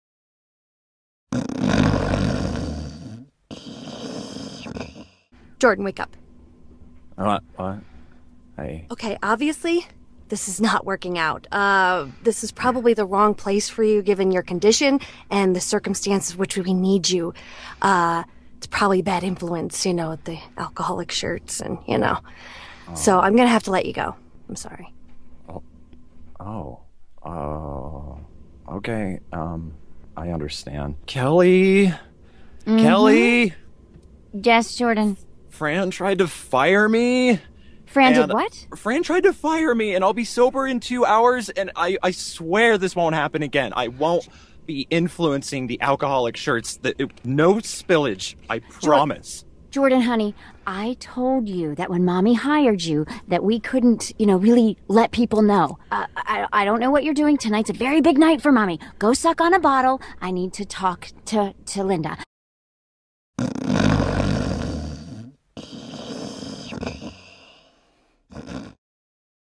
CLIPS You're Fired !!
Jordan, Fran and Kelly are given status assignments and the phrase "alcoholic t-shirts" to use in a scene. Apparently, someone gets fired!

Group Workout clip from VO-Improv students... All improvised, of course, during a group improv audio-only workout held by LovethatImproVO.com, the only online audio-only training teaching and practicing improv for voice actors.